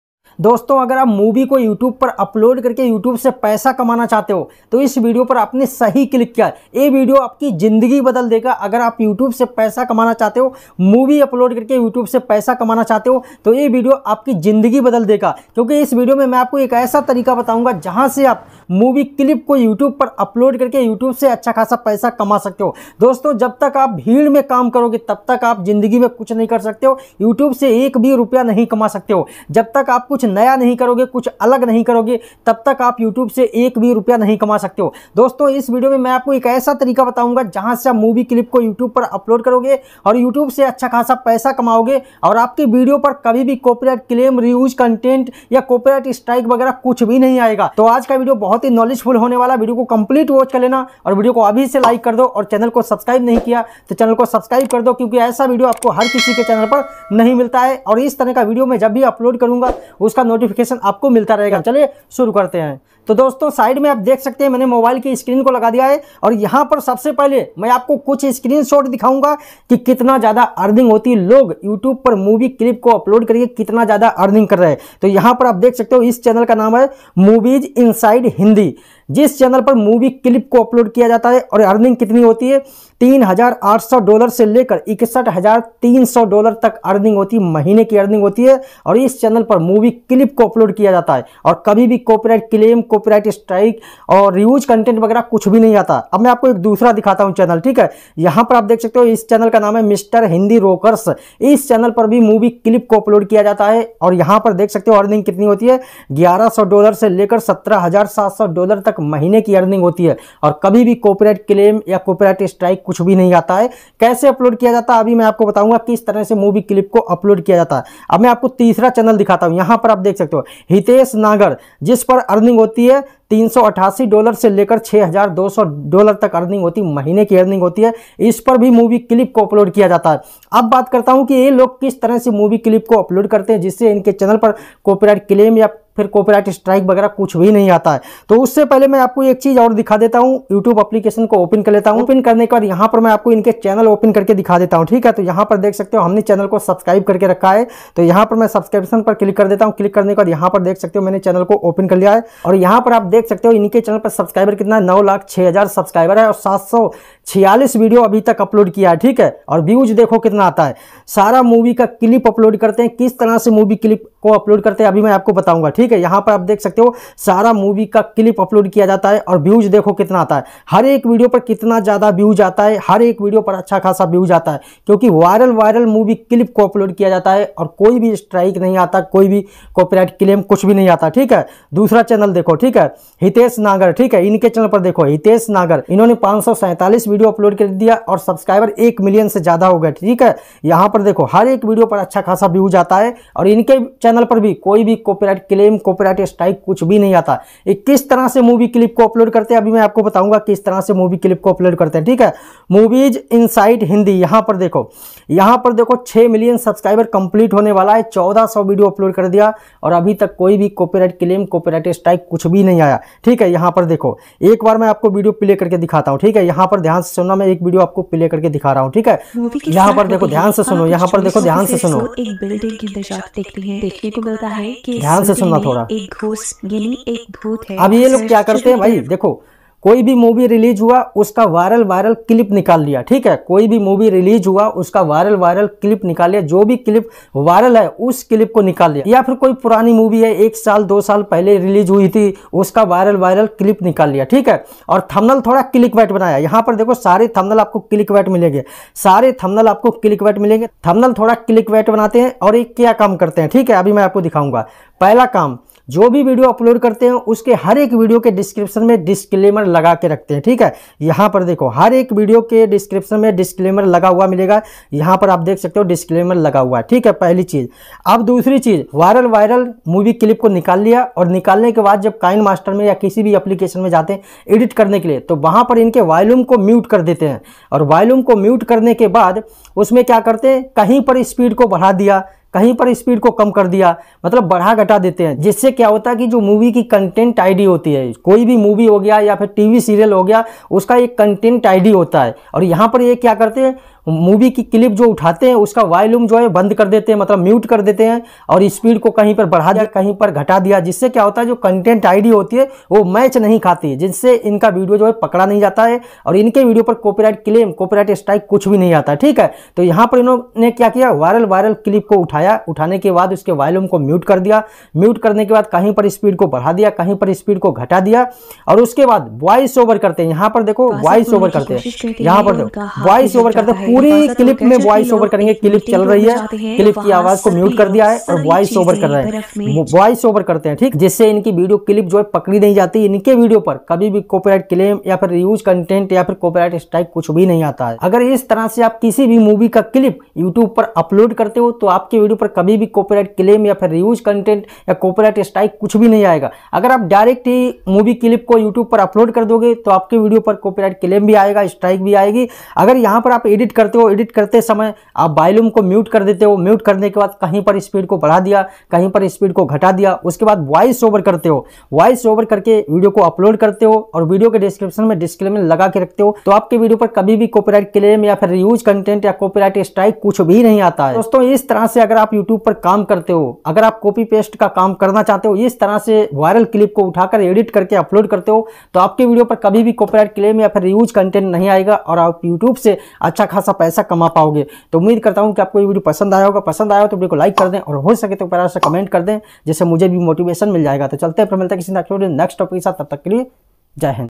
🤑(100%) Movie Upload Karke Paise Kaise Kamaye || YouTube Per Movies Upload Karke Paise Kaise Kamaye
About This Video👇
doston humne is video mein bataya hai ki aap YouTube per movie clip ko upload Karke paise Kaise Kama sakte hain aur aapke video per kabhi bhi koi bhi copyright strike, copyright claim kuchh bhi nahin aaega isliye video ko complete watch kar lena

Jagveer Chauhan

From - Shahjahanpur UP ( India )